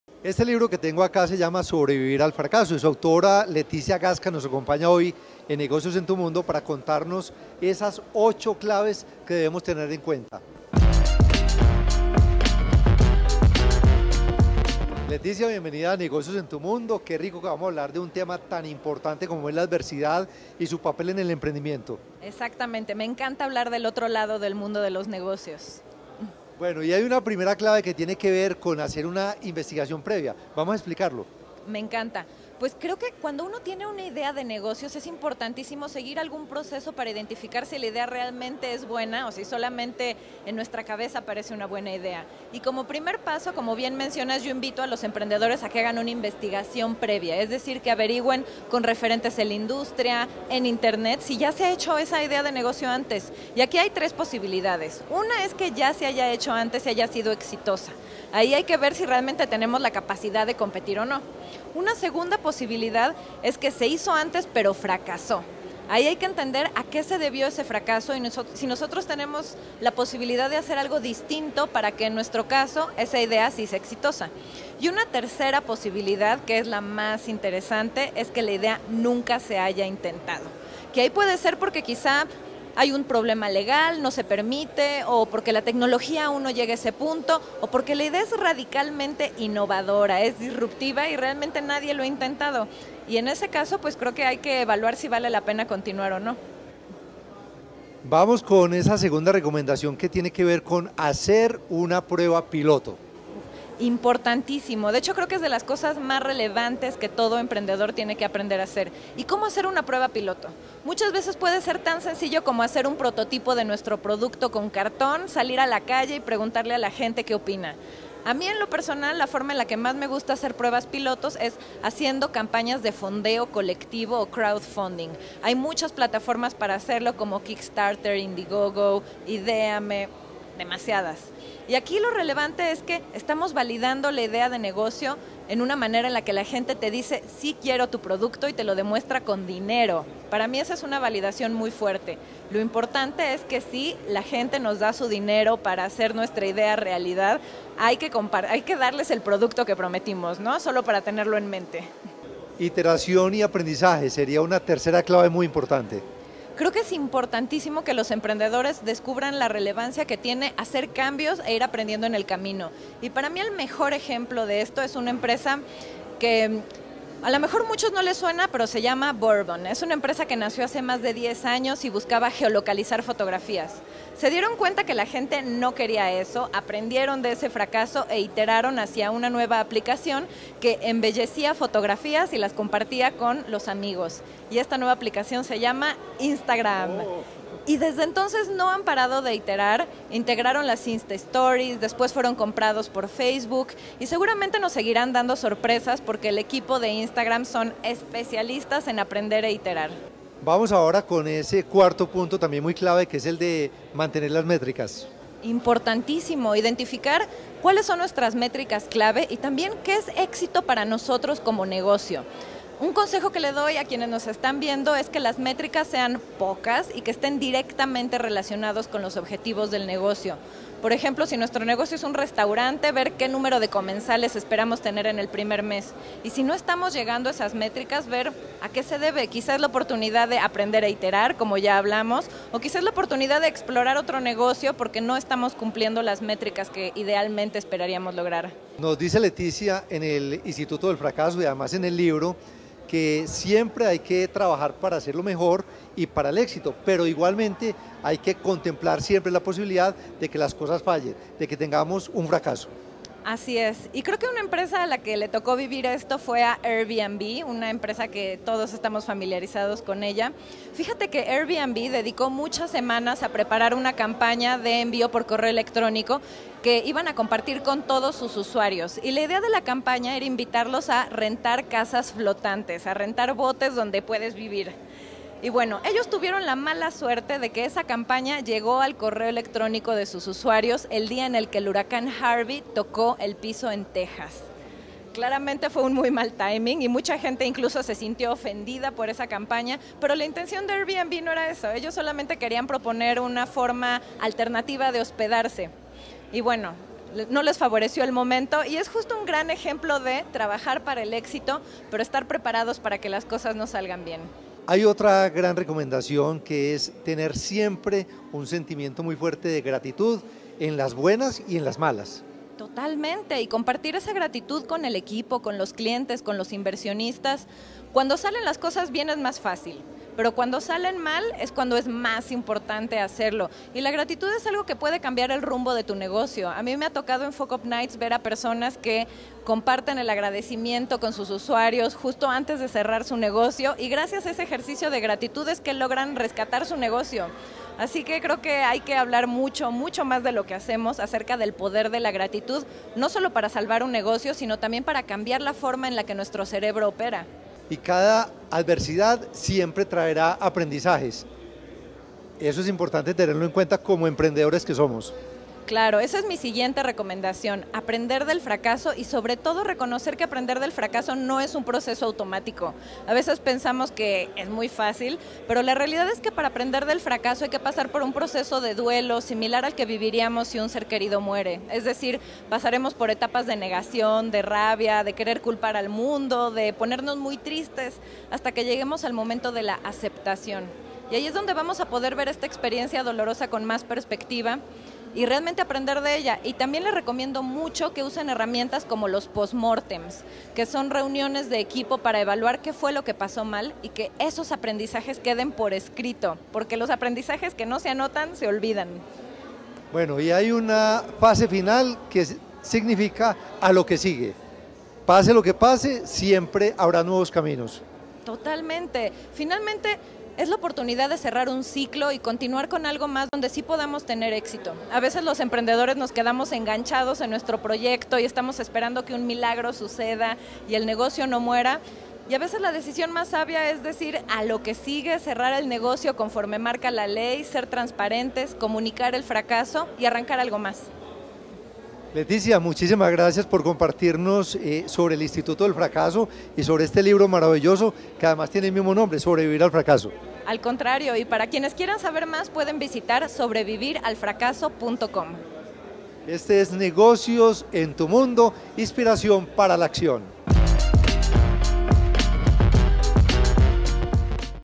La adversidad y su papel en el emprendimiento - [Negocios en Tm] ®
Partiendo del libro Sobrevivir al fracaso, su autora Leticia Gasca,
directora ejecutiva del Instituto del Fracaso, nos habla de 8 claves para aplicar a la hora de emprender.

En esta entrevista nuestro director, Juan Carlos Yepes, conversó con la autora del libro sobre este interesante tema.

Negociosentm